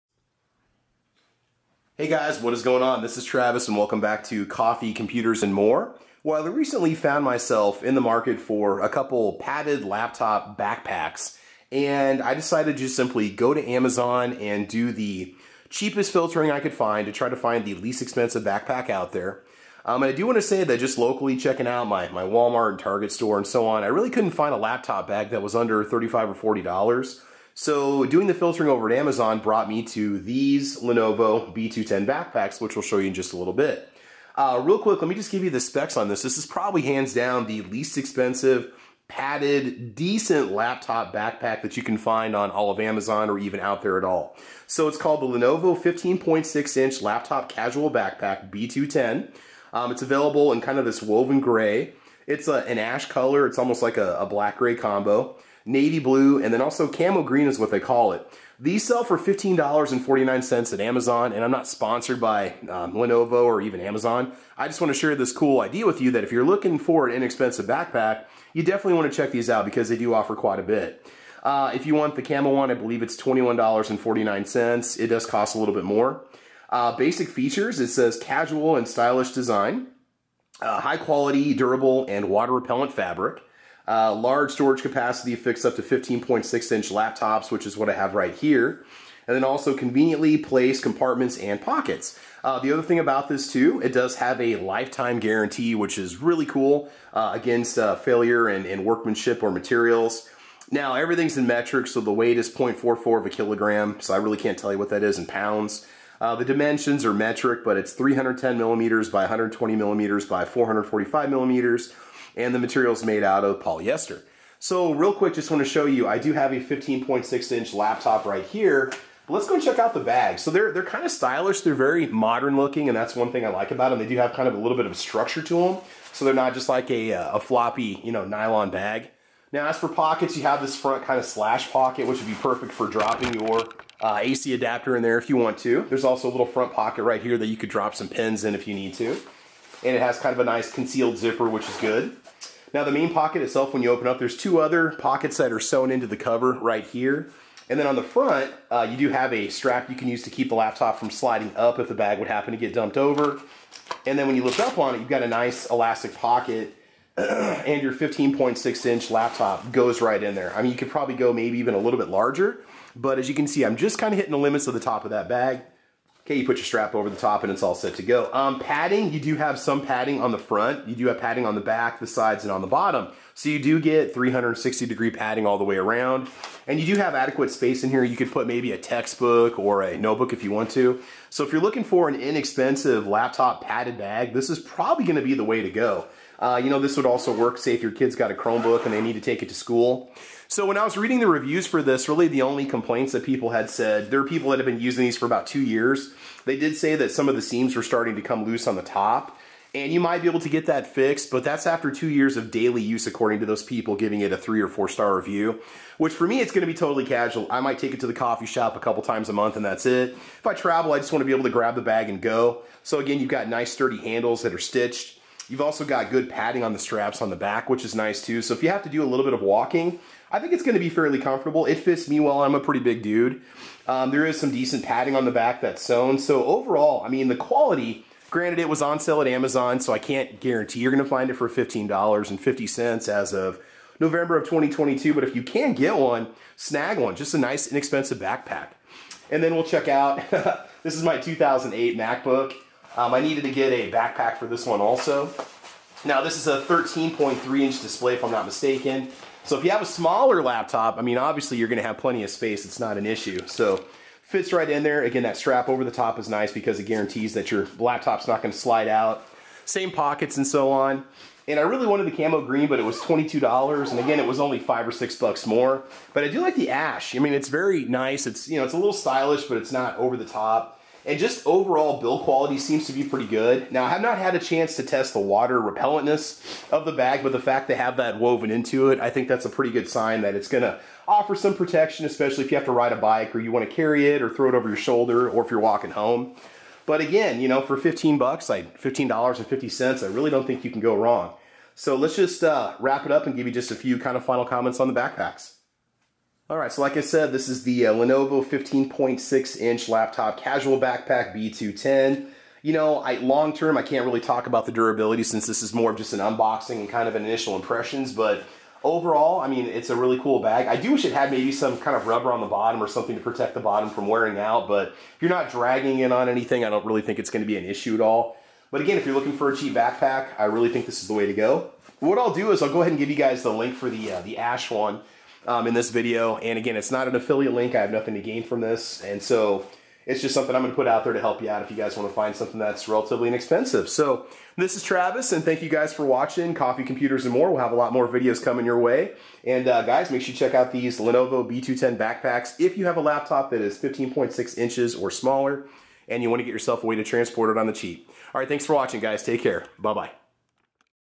Lenovo B210 padded 15.6" laptop backpack...computer protection on the cheap!
At less than $16 over at Amazon.com as of November of 2022, the Lenovo B210 backpack offers a bunch of great features at a very low price.  Let's take you though the specifications and overview of this computer bag!